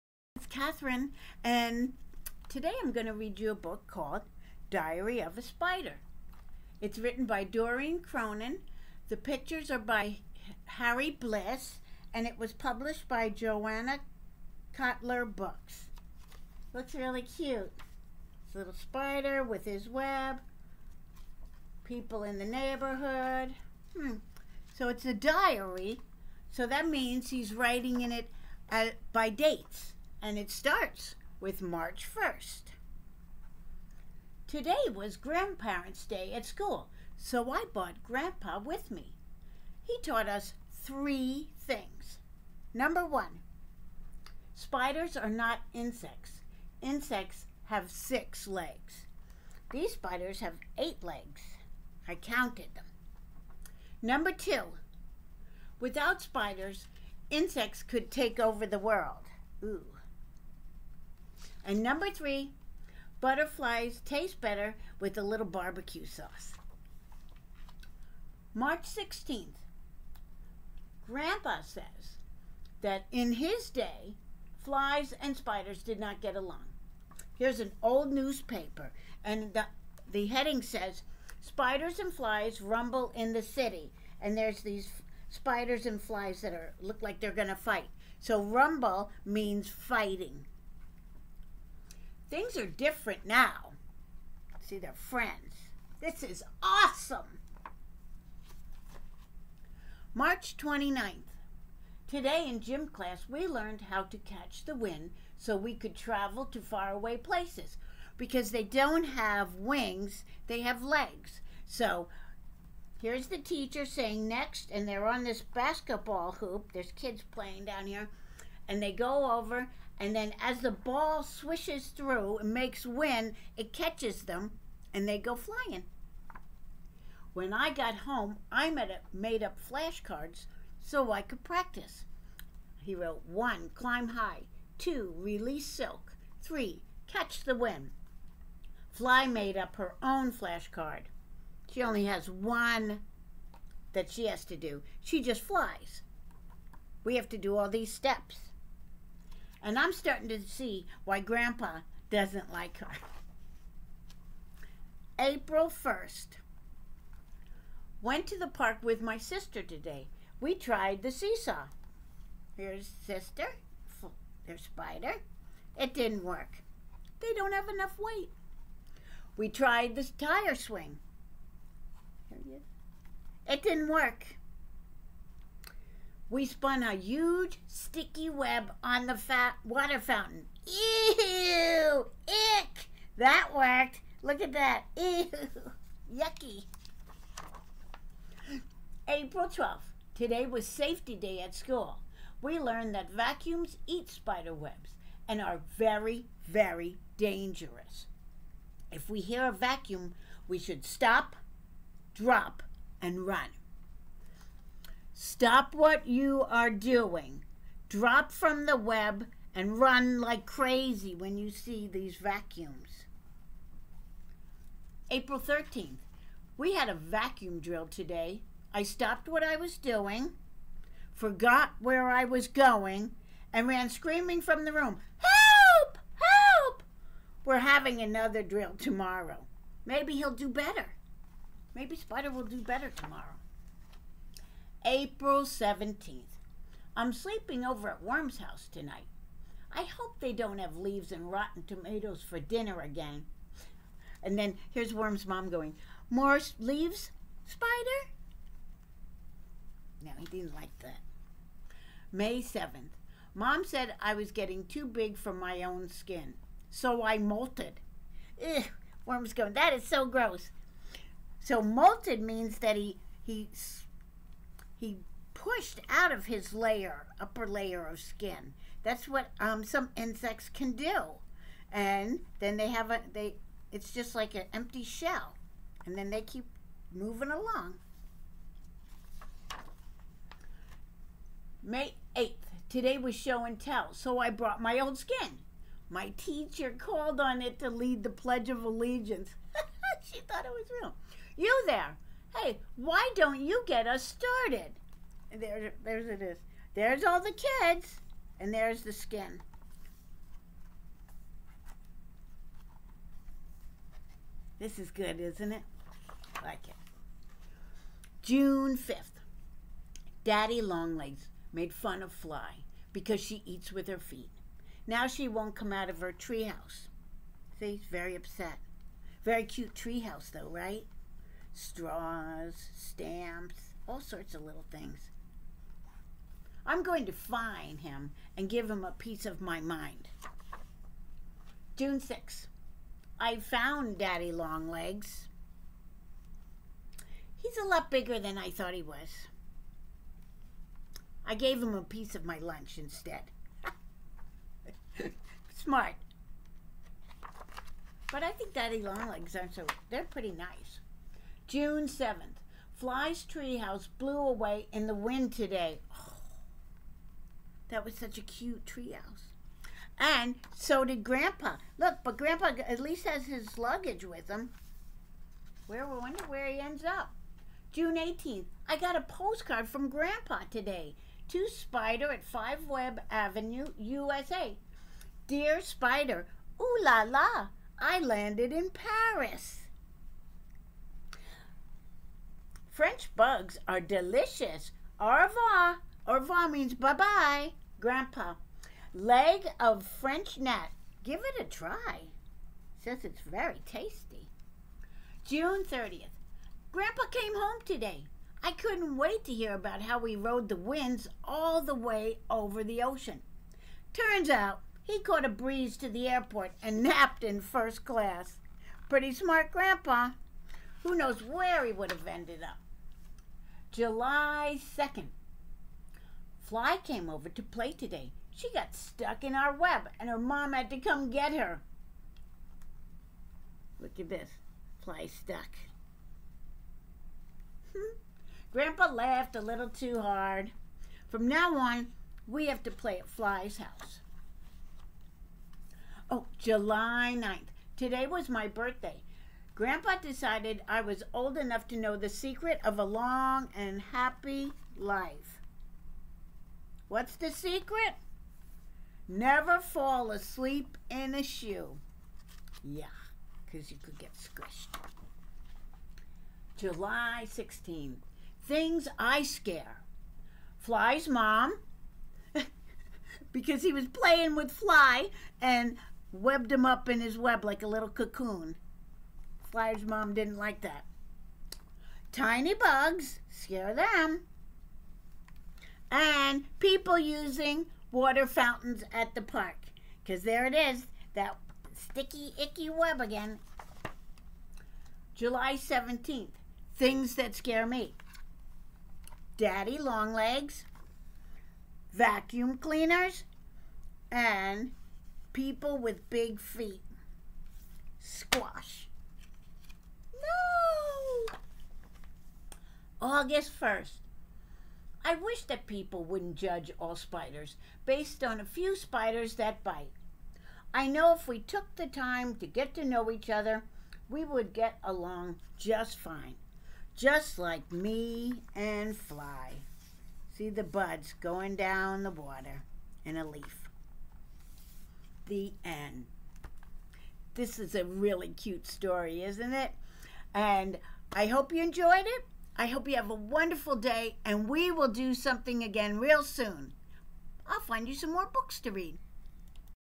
Diary of a Spider by Doreen Cronin -Story Time
Miss Catherine reads "Diary of a Spider" by Doreen Cronin.